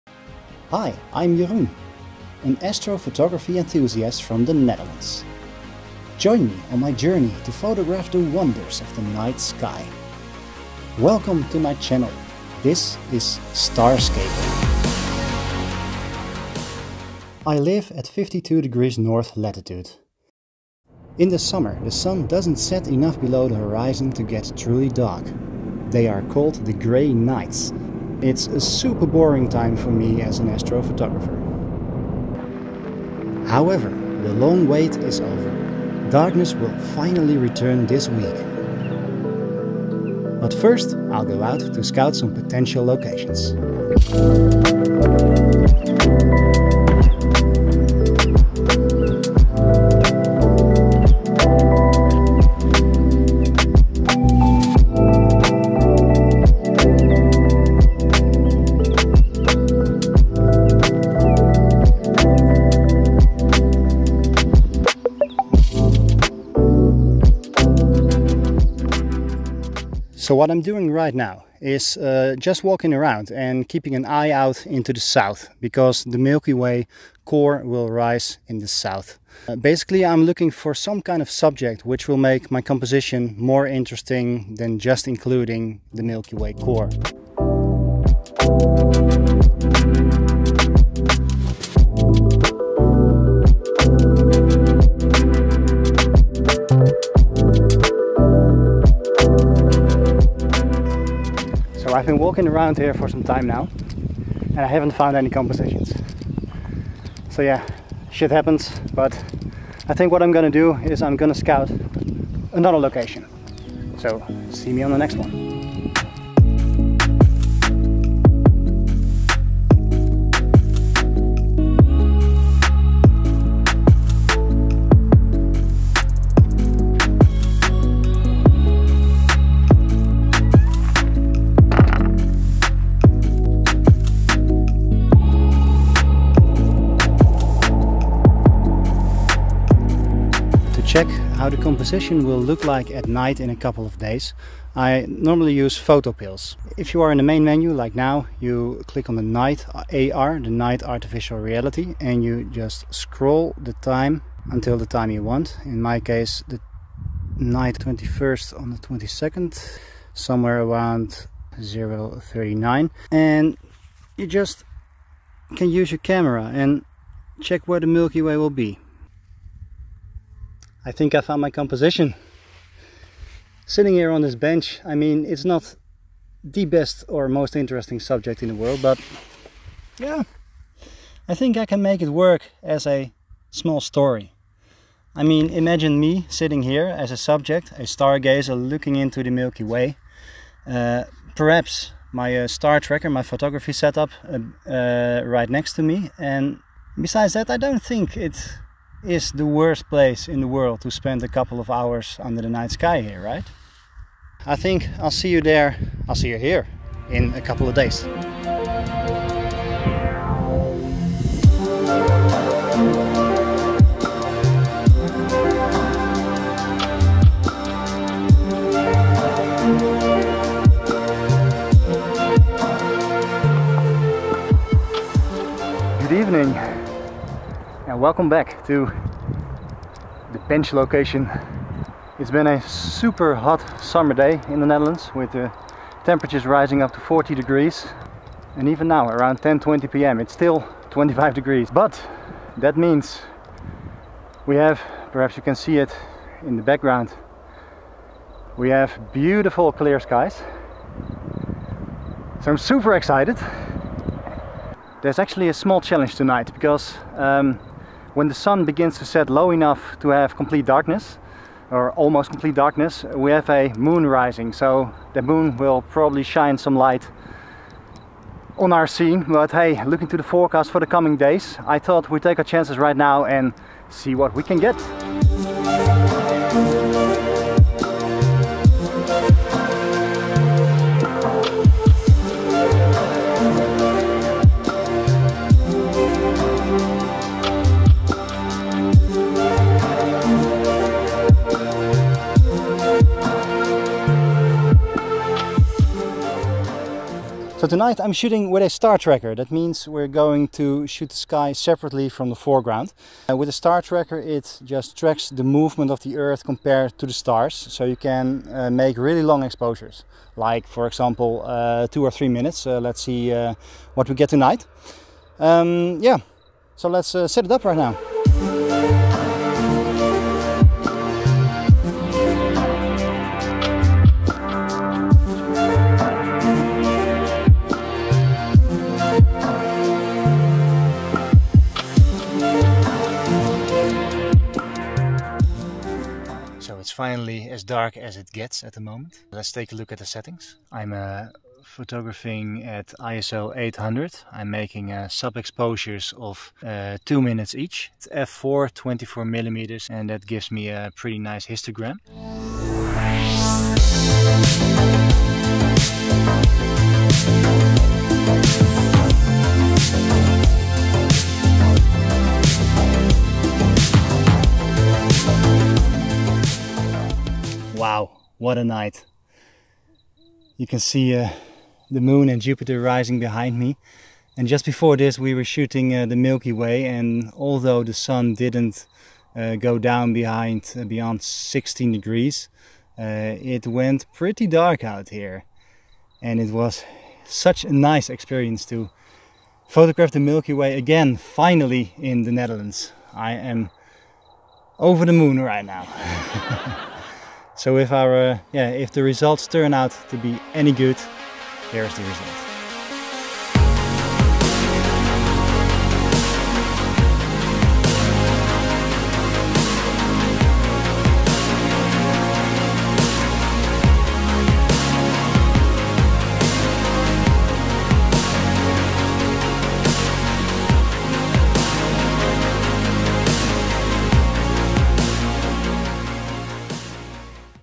Milky Way Photography in The Netherlands
After months without complete Darkness I head out into the Night to Photograph the Milky Way. Join me on my Astrophotography Adventure.

(Dutch below)
Na maanden zonder echte duisternis ga ik erop uit om de melkweg te fotograferen in Nederland. Ga je mee op dit astrofotografie avontuur?